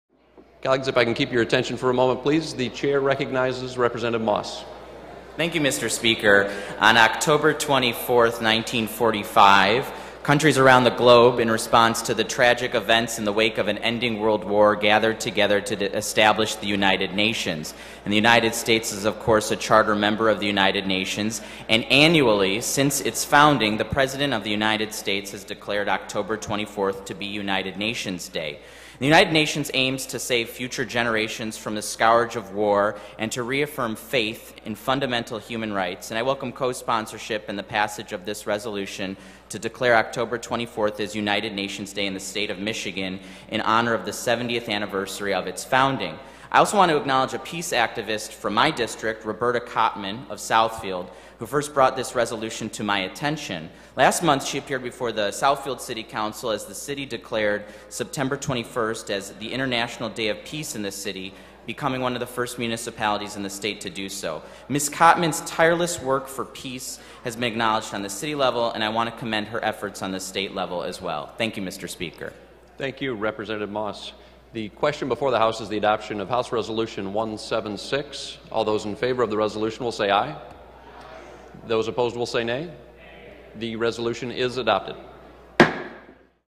Rep. Jeremy Moss Passes United Nations Day Resolution
Rep. Jeremy Moss passes a resolution to declare October 24, 2015 United Nations Day in the state of Michigan. Rep Moss also recognizes the great work of peace activist Roberta Cottman of Southfield.